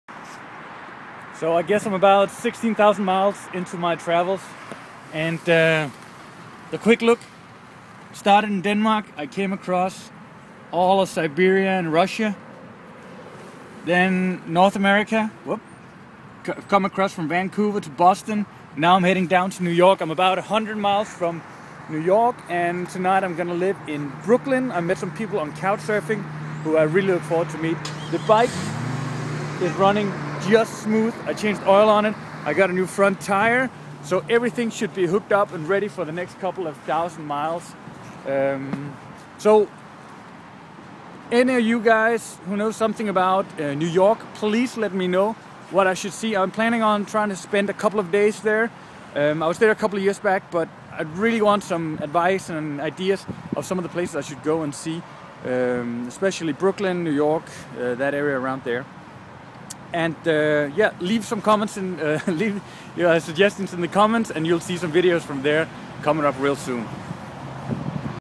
100 miles from New York
Slowly making my way south after going east for 16000miles:-)